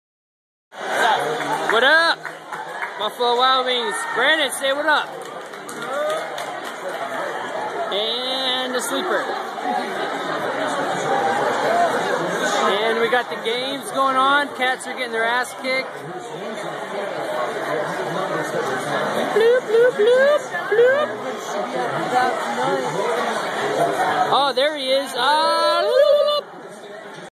Passed out i a crowd of people.
This video was uploaded from an Android phone. At a bar watching the Arizona Wild Cats play in a NCAA Football bowl game in 2010. two red bulls from the table next to us didn't help.